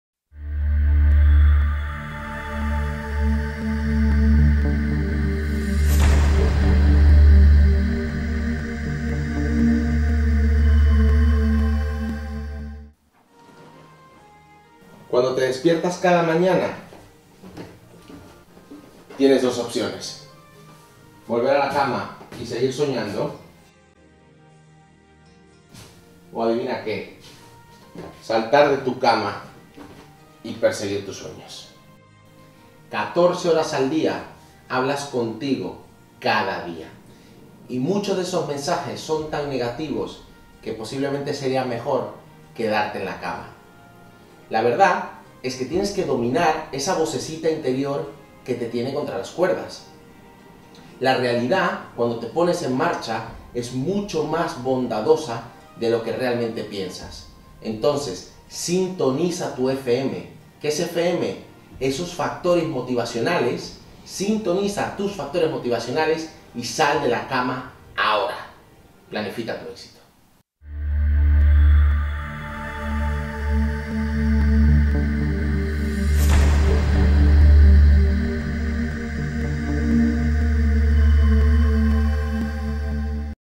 Persigue tus sueños - Planifíca tu éxito con Jose Manuel Torres
Cuando te despiertas cada mañana tienes 2 opciones: Volver a la cama y seguir soñando. O Saltar de tu cama y PERSEGUIR TUS SUEÑOS.
14 horas al día estas hablando contigo mismo. Y en muchas ocasiones son mensajes que seria mejor quedarte en cama. Acostumbrate a dominar esa vocecita interior que en muchos casos te tiene contra las cuerdas.
Muchas veces asumimos hechos que no son ciertos. La verdad es que la realidad es mucho mas bondadosa de lo que la gente cree.
SINTONIZA tu FM: tus Factores Motivacionales y SAL DE LA CAMA AHORA!!!